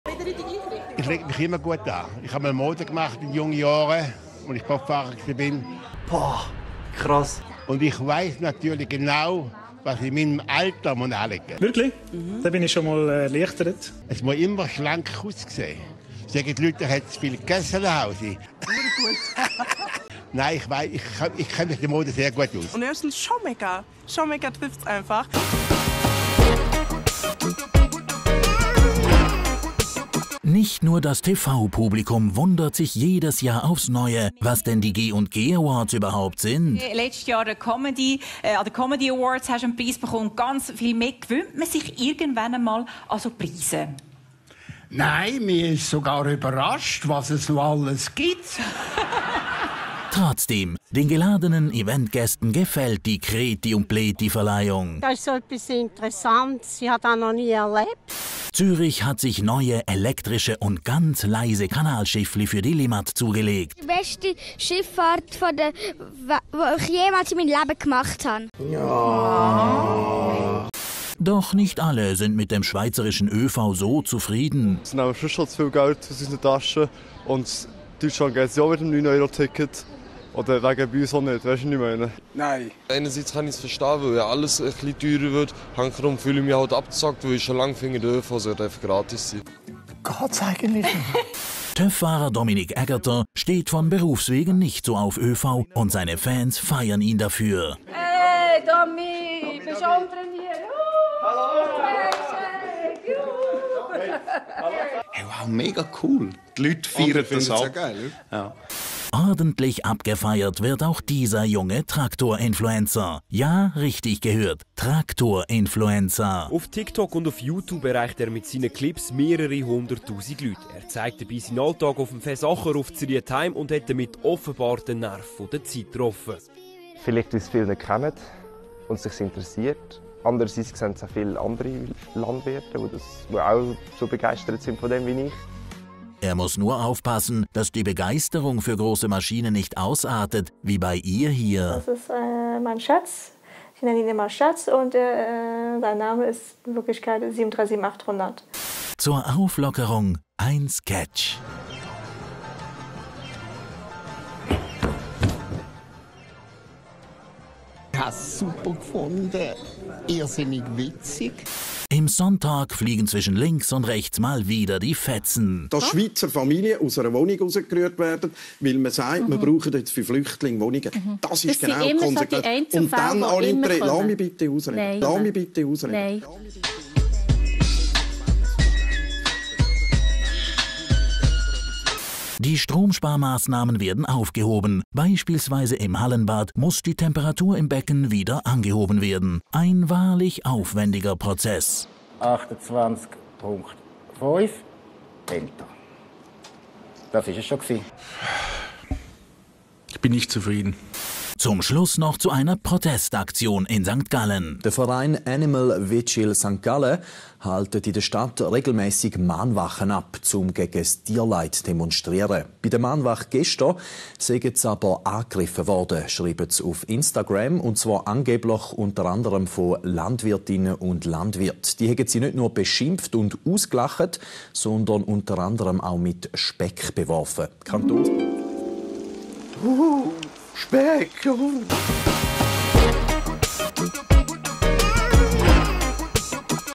Folge 5: Bobfahrer, Modegott & Traktor-Influencer 🥰 - 👉 Abonniere @zappalot 👈 - Best of TV-Weekend
☝️☝️☝️ Follow @zappalot now!!! ☝️☝️☝️ - Die besten TV-Momente des Wochenendes immer am Montag im Best of TV-Weekend by Zappalot

Folge 5: Bobfahrer, Modegott & Traktor-Influencer 🥰

Die Zappalot_Crew dankt allen TV-Sendern, -Moderatoren & -Machern für ihren Beitrag, insbesondere unseren geliebten Schweizer TV-Kanälen